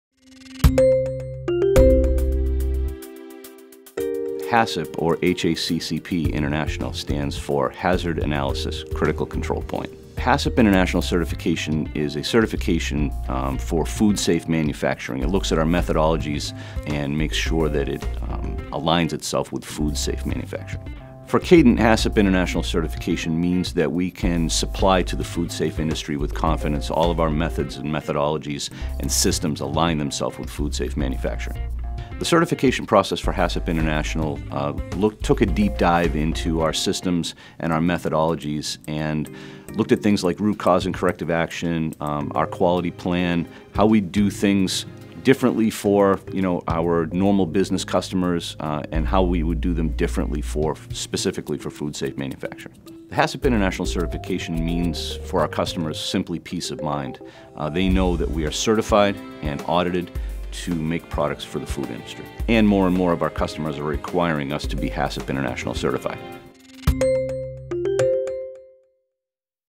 HACCP International Certification and Kadant Solutions
HACCP International provides certification for products, materials, and services brought into food processing and handling which meet best practices in terms of food safety. Join Jeff Bowser, Manufacturing Manager for Kadant Solutions, to learn what this certification means to our customers in the food industry.